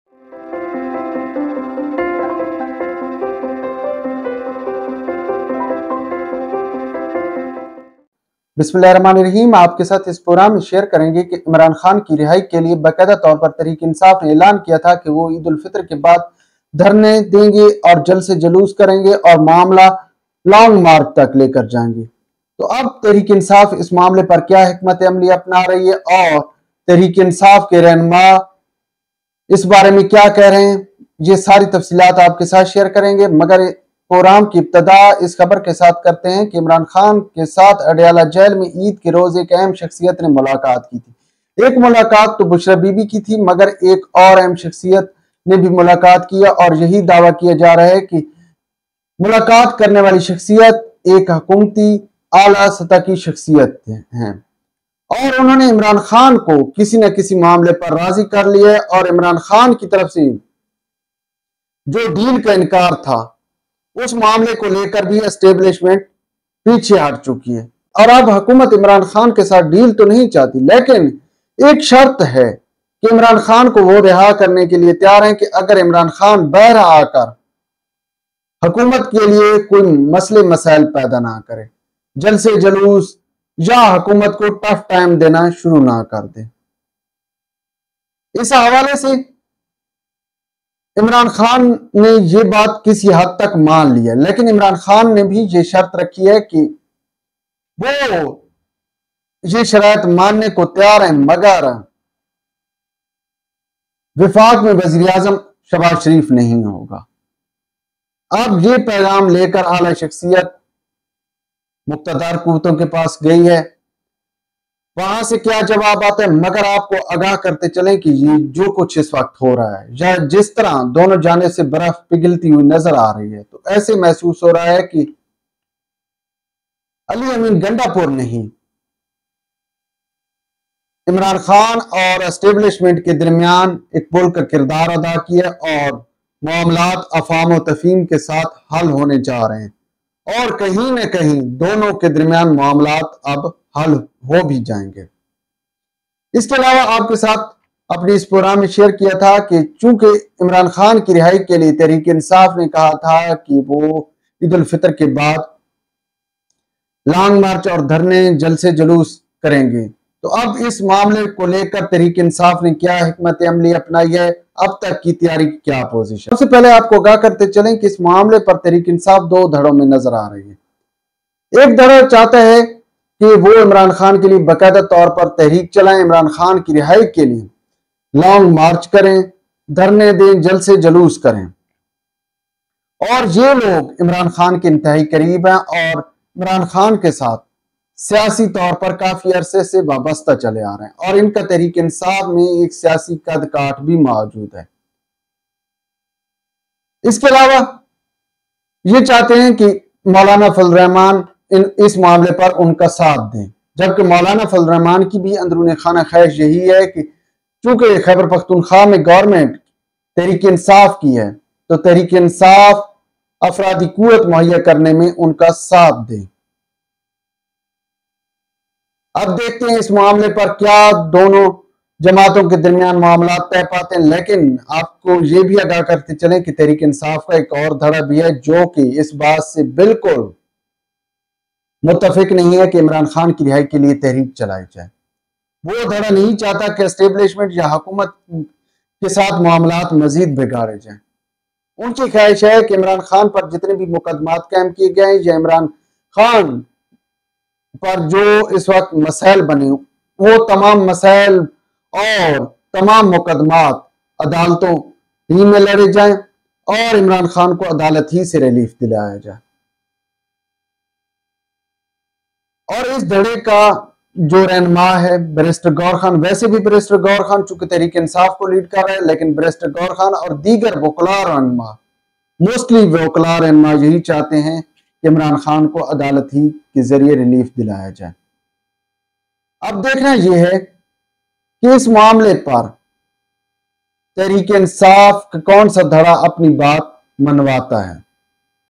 Imran Khan's Release | Political Divide and Future of PTI
"In this video, I discuss the recent meeting between Imran Khan and a prominent figure on the day of Eid, where discussions of potential deals between Khan's government and other parties surfaced. Additionally, I delve into the internal divide within PTI regarding Khan's release, with one faction led by Pervez Khattak advocating for legal resolutions, while another faction prefers a more confrontational approach. Join the conversation as we analyze the complexities surrounding Imran Khan's potential release and the implications for PTI's future.